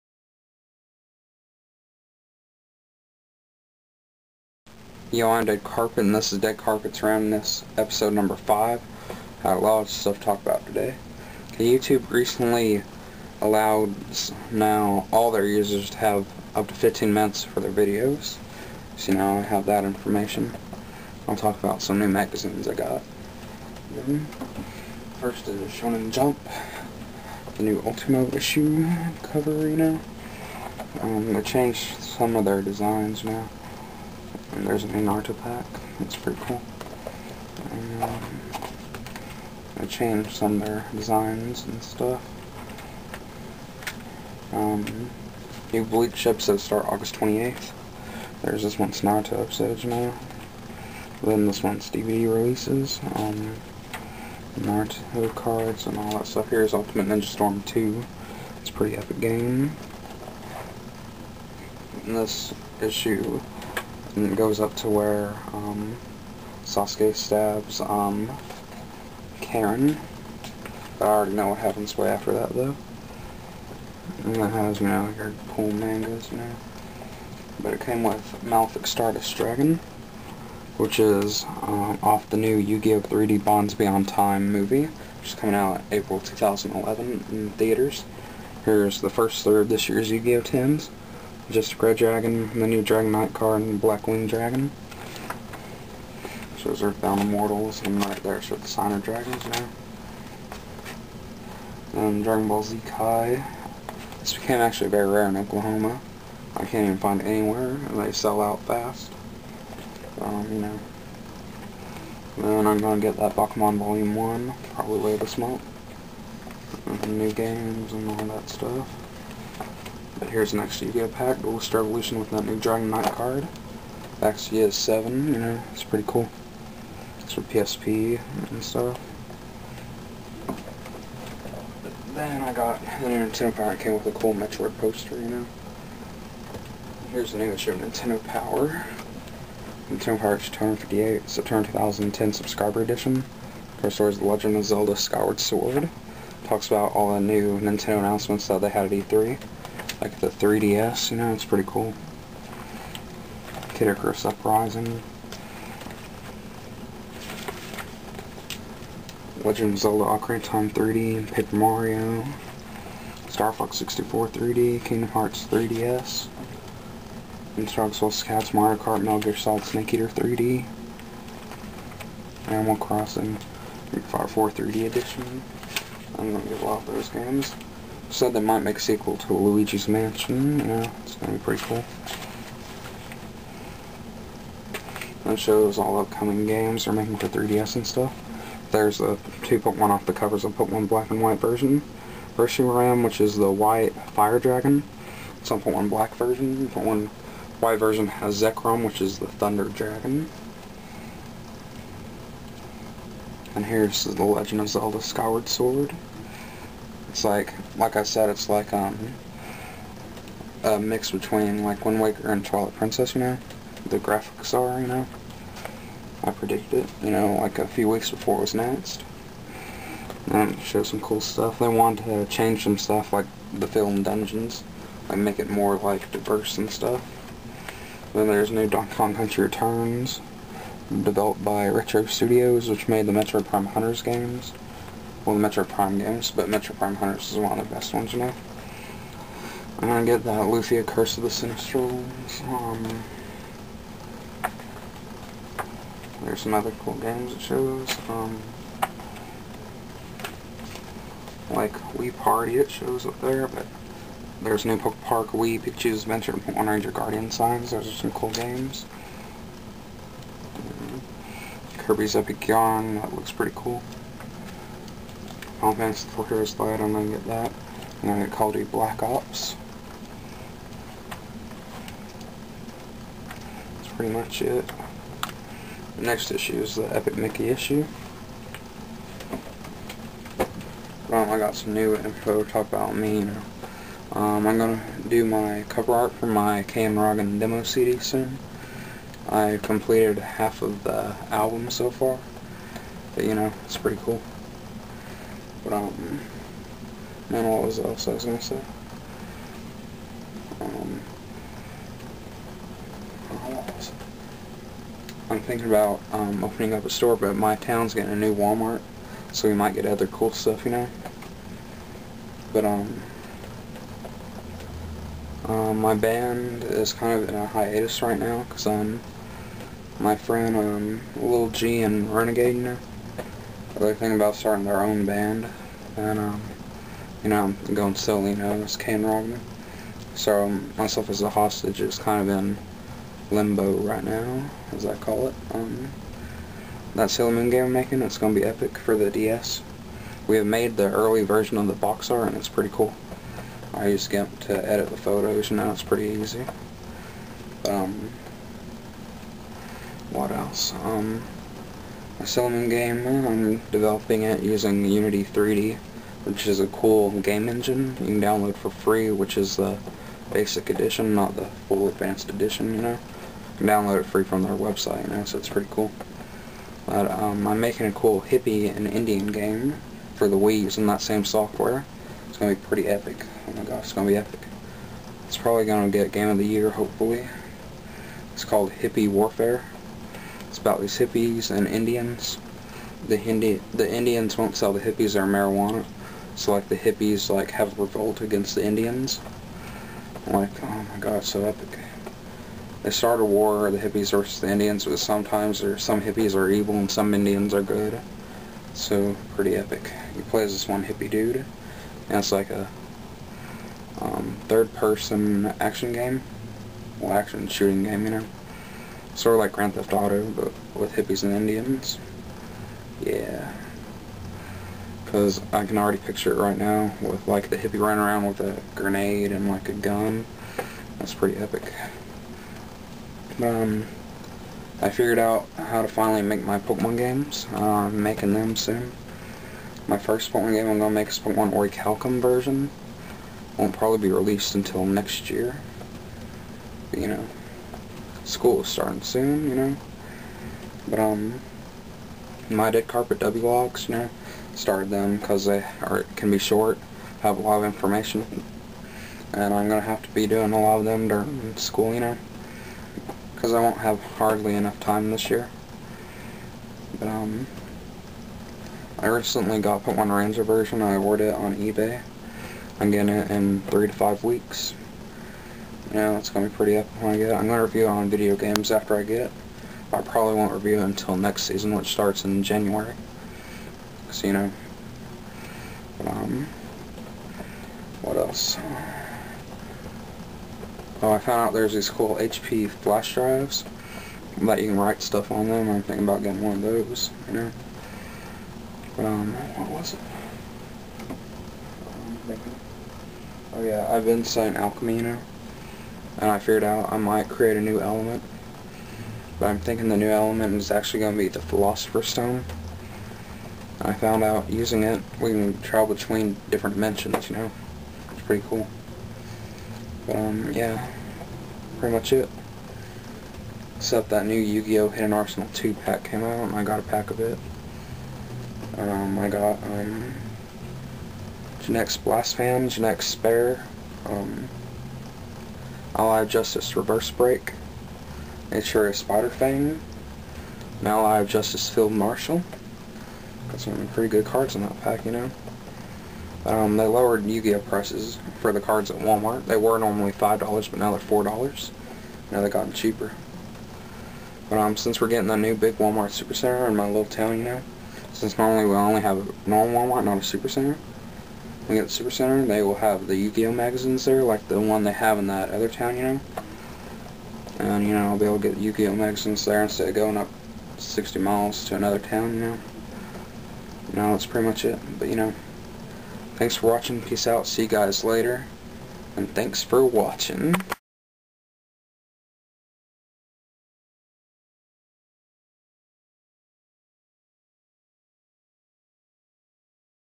Deadcarpet's Randomness Episode #5
I have a lot of new info in this episode. On Youtube, you can now have up to 15-minute long videos. I show my new Shonen Jump. It came with Malefic Stardust Dragon. Dragonball Z Kai is starting to become rare in Oklahoma. In the new issue of Nintendo Power, they showed a lot of new 3DS games. I'm making a lot of cool games. My band is in limbo.